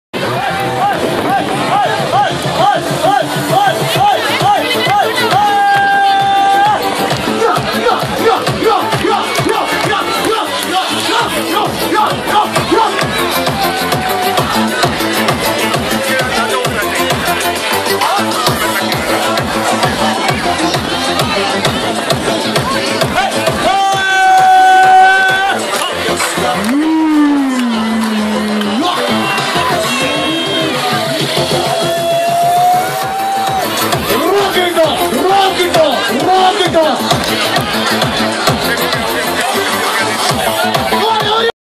Holi 2013 celebrations busan, korea IV
You wont see such a holi celebrations even in India.. and this is beauty of holi which can unite anyone...doesn't matter which caste, religion, or place.